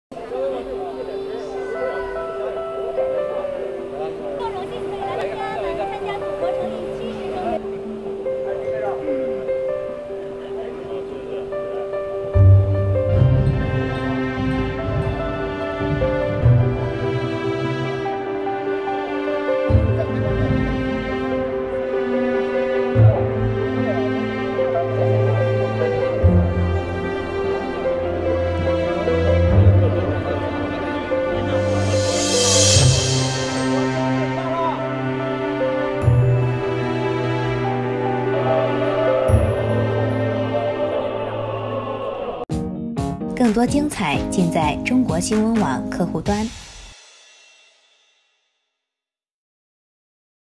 看不到阅兵的志愿者 观礼台背后只为服务他人观礼
10月1日国庆阅兵当天，在观礼台背后的几位志愿者为现场观众提供问询等服务，并不时帮助观众拍照留念。为了服务他人观礼，身在天安门现场的他们却成了看不到阅兵的一个特殊群体。